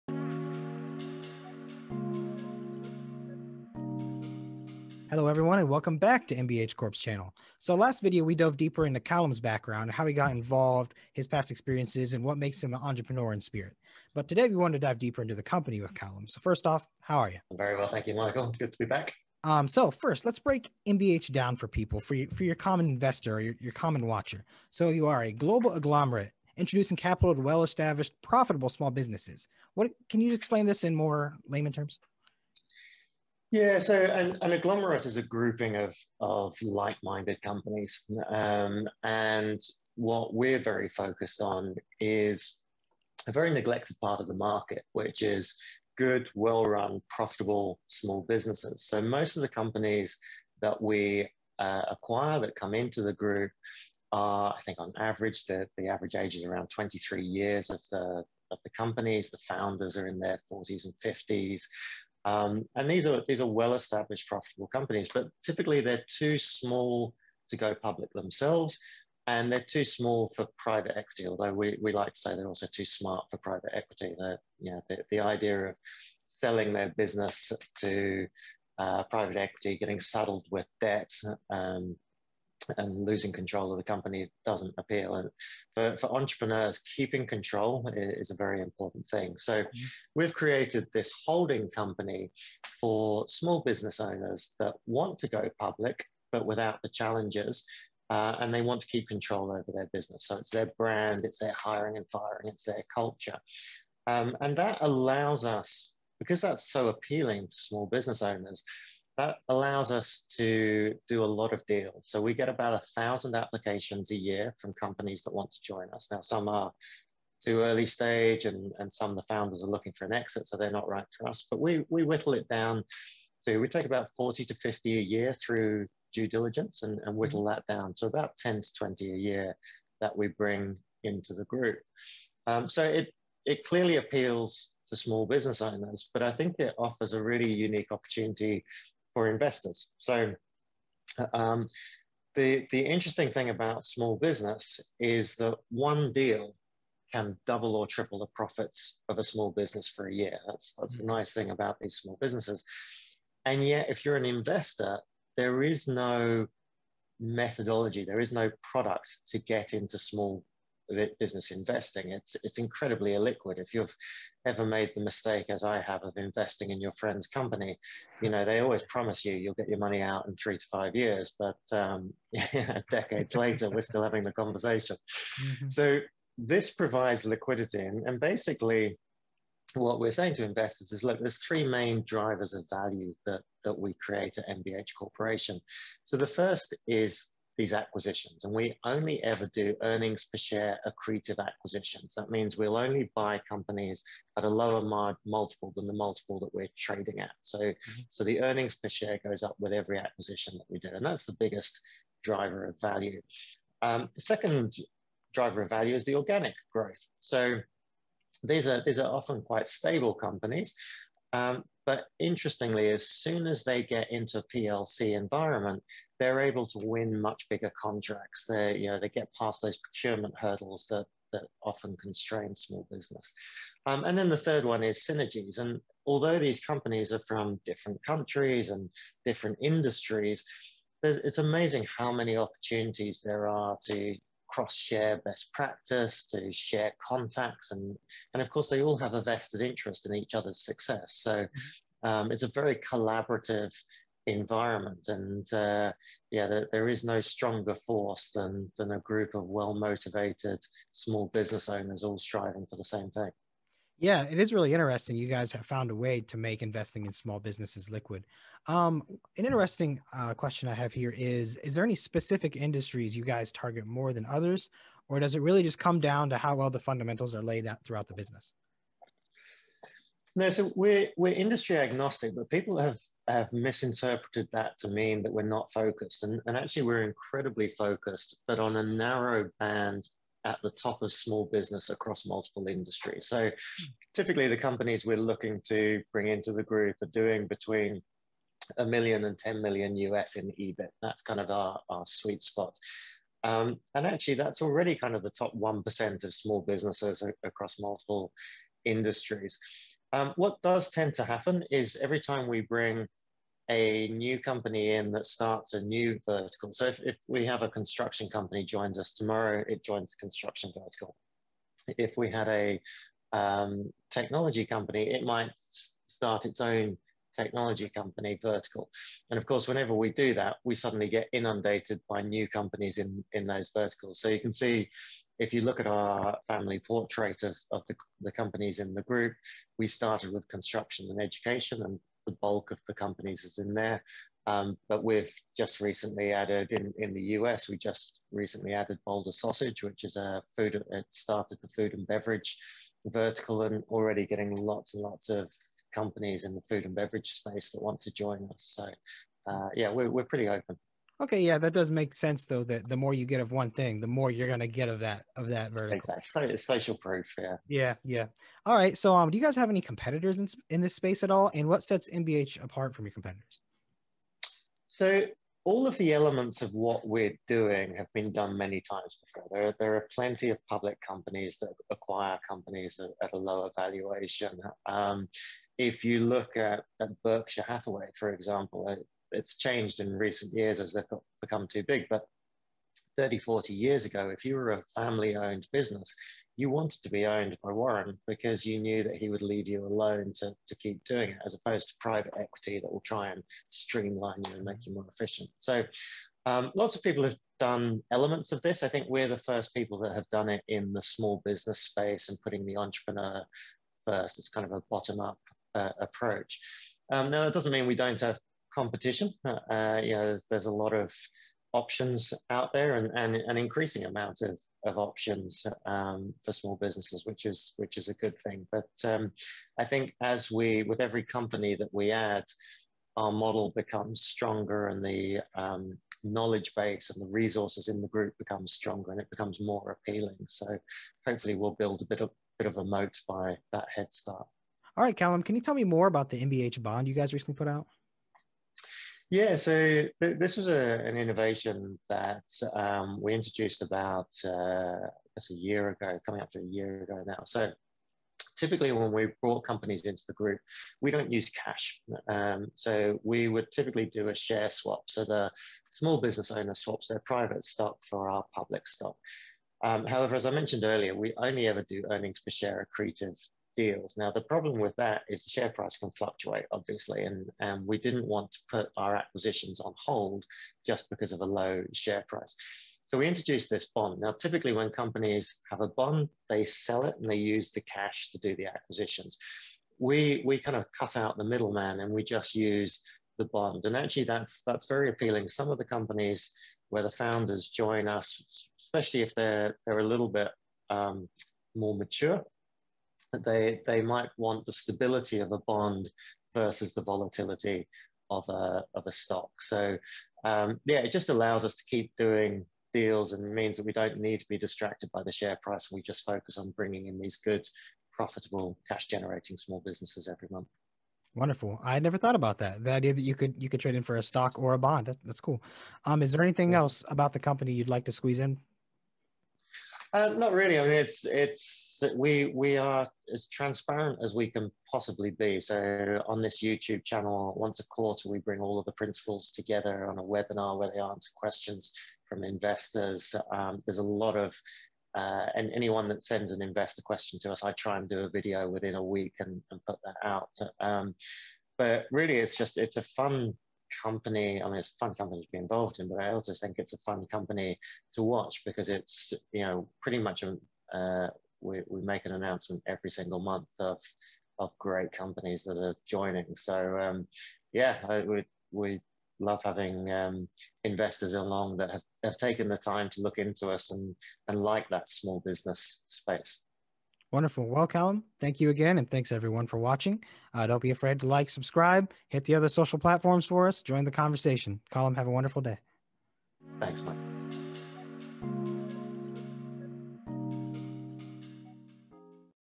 Callum Dives Into MBH Corporation's Structure and Mission
MBH is a holding company for small business owners that want to go public but keep control of their business, their brand and their culture.